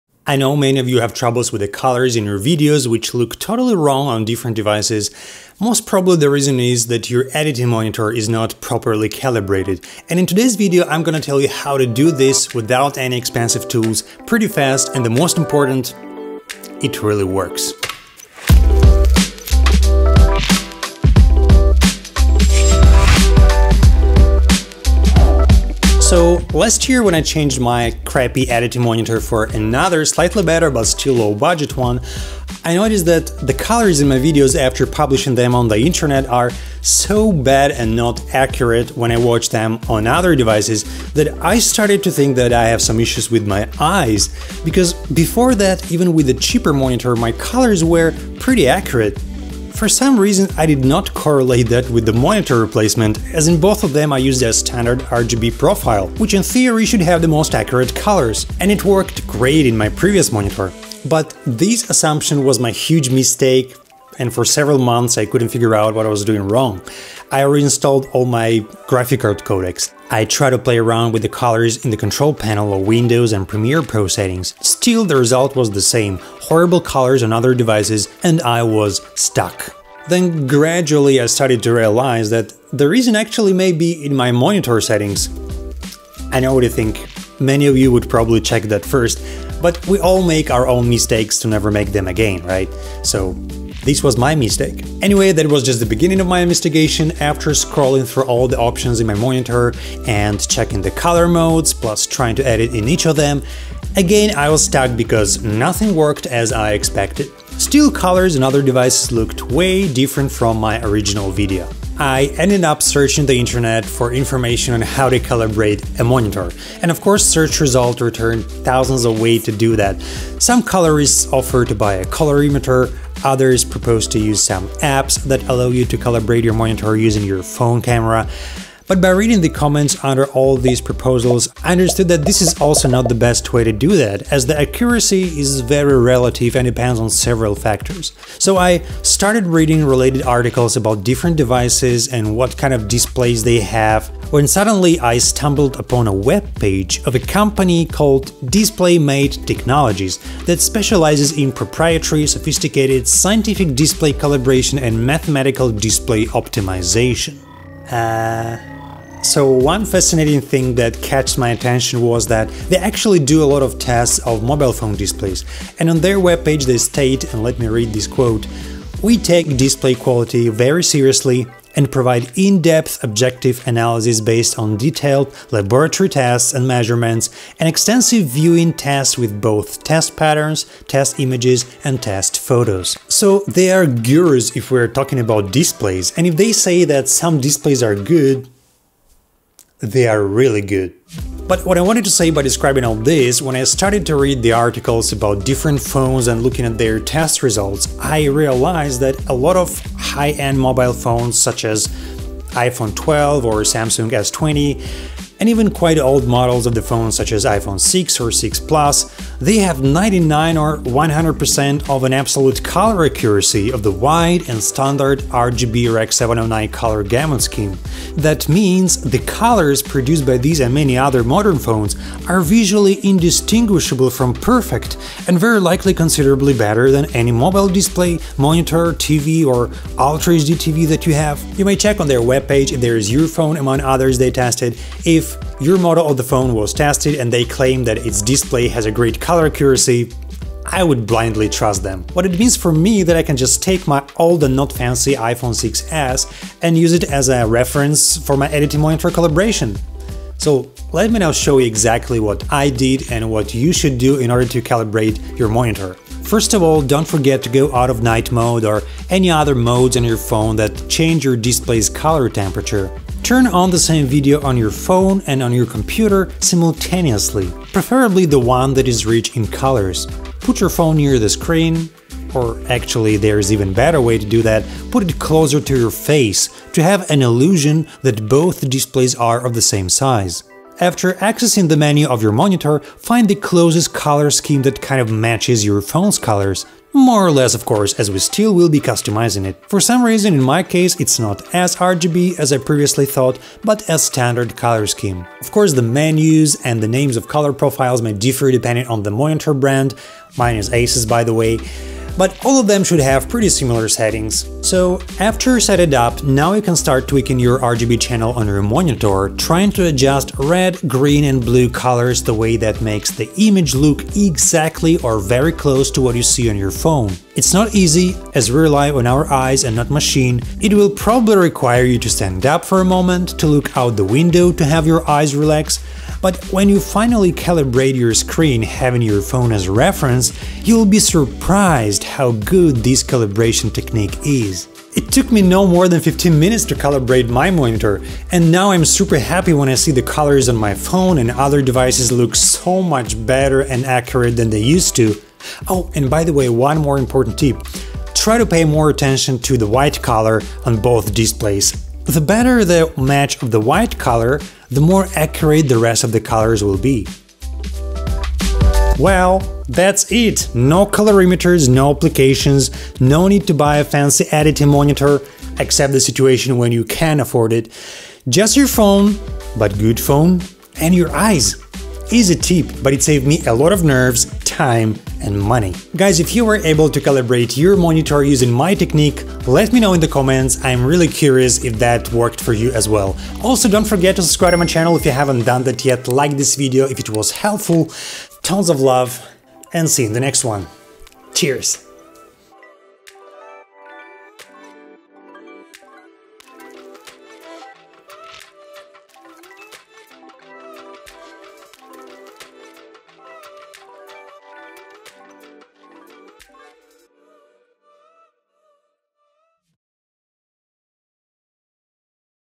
I was speechless!.. How to CALIBRATE a MONITOR without a colorimeter
In this video I show how you can easily calibrate your monitor without using a Colorimeter or mobile applications. This technique is not perfect and depends on the model of the phone you own, however it works great for me.

Here is a very budget ASUS 23'' monitor you saw in this video
Here you can see some tests for the Absolute Color Accuracy

✅ Kindle/eBook format